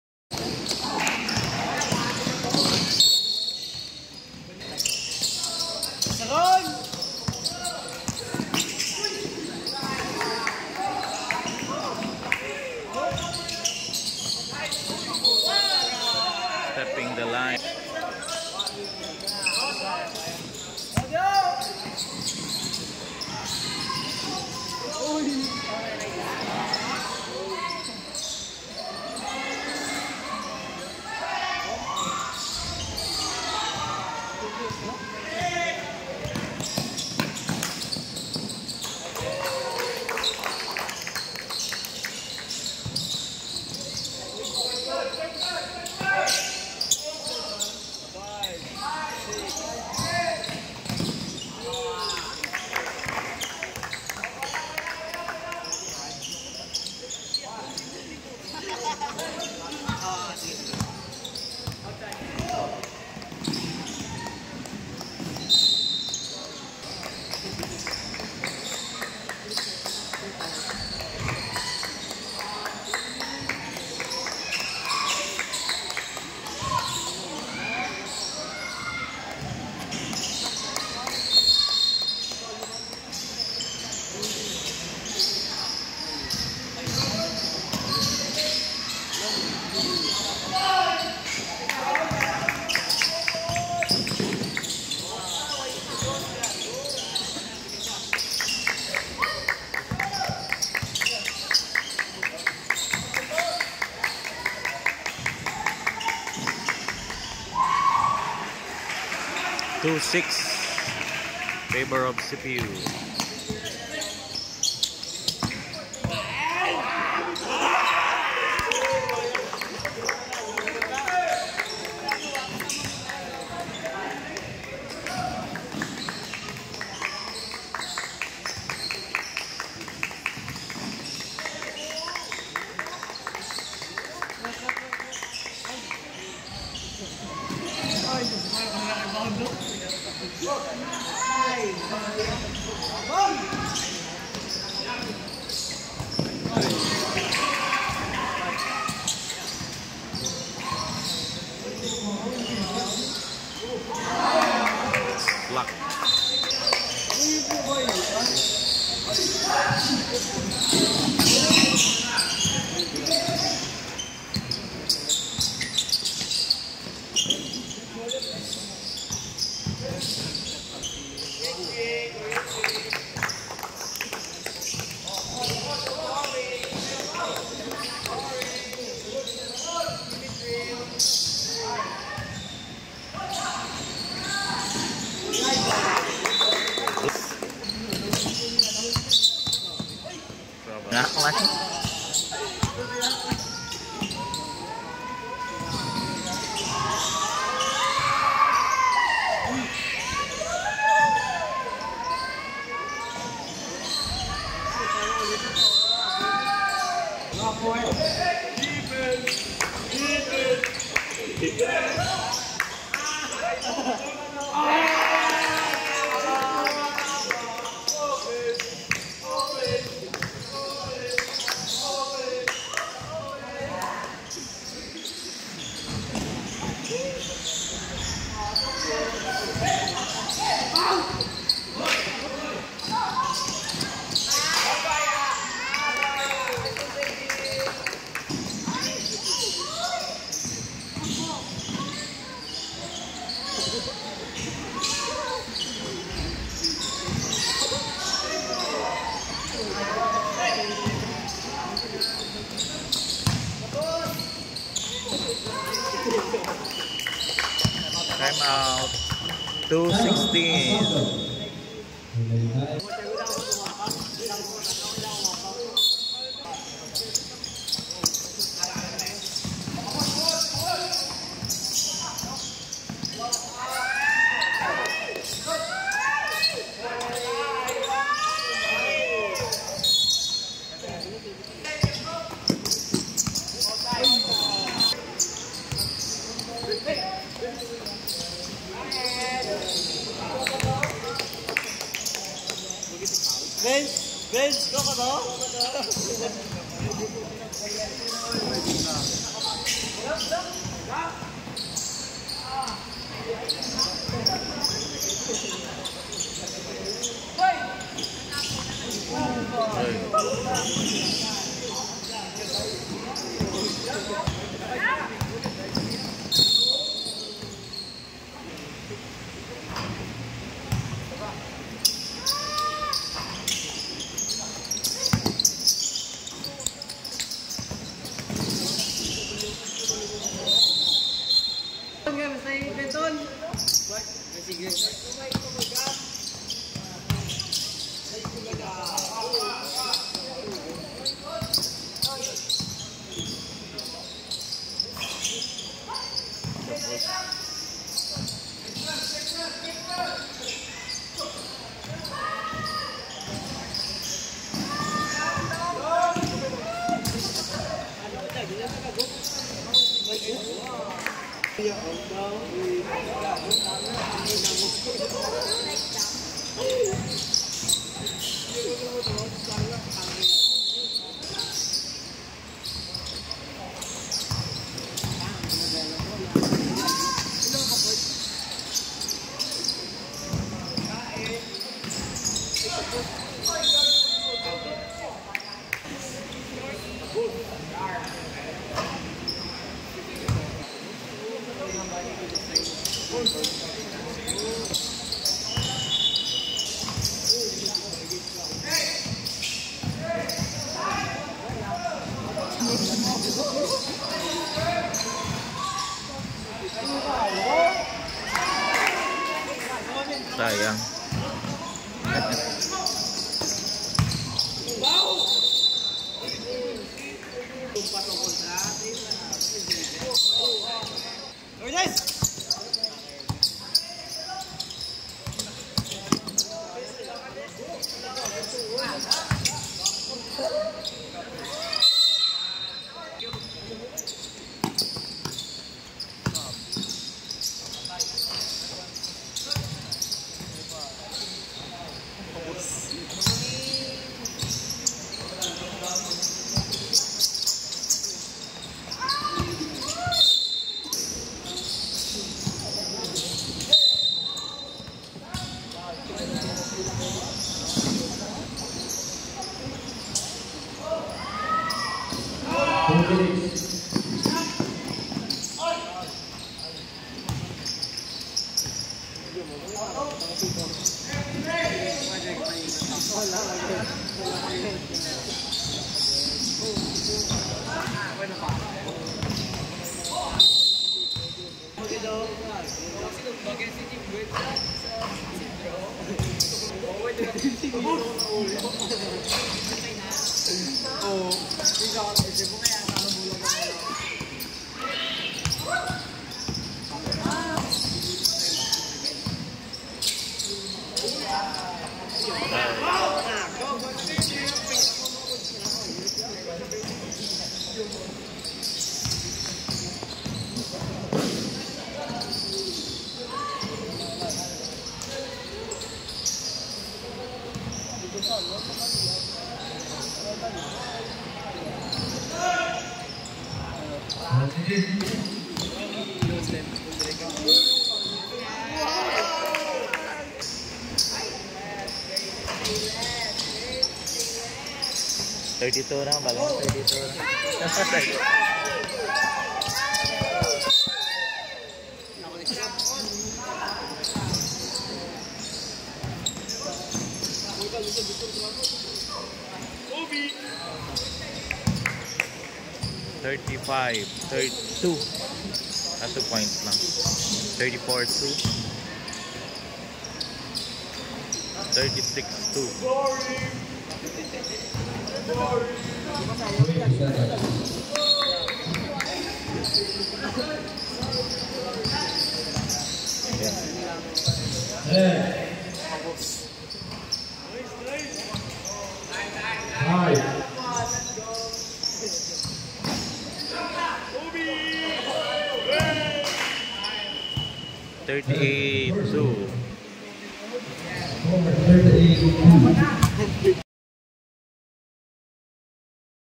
CPU Golden Lions holds off Assumption | First Quarter | PRISAA 2024
Another year, another adventure! Another chance to praise the Lord!
Come and watch the Second Quarter Basketball Match between Central Philippine University High School and Assumption Iloilo of the PRISAA Games at Angelicum Gym last February 23, 2024.